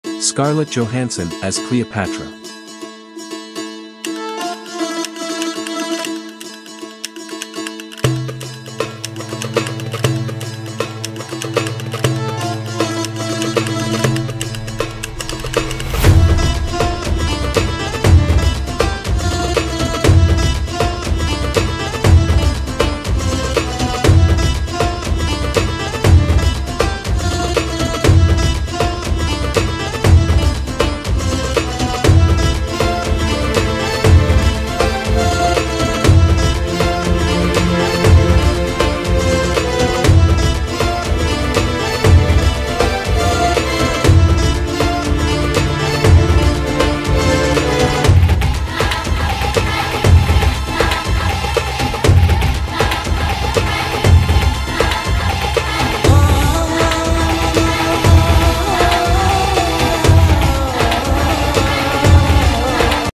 Scarlett Johansson as Cleopatra
"Explore the mesmerizing portrayal of Cleopatra by the talented Scarlett Johansson in our latest video! Join us as we delve into the captivating world of ancient Egypt, where Johansson brings to life the legendary queen with her grace, charm, and unparalleled talent. Discover the intrigue, drama, and power of one of history's most iconic figures reimagined through the lens of modern cinema. Don't miss out on this epic journey!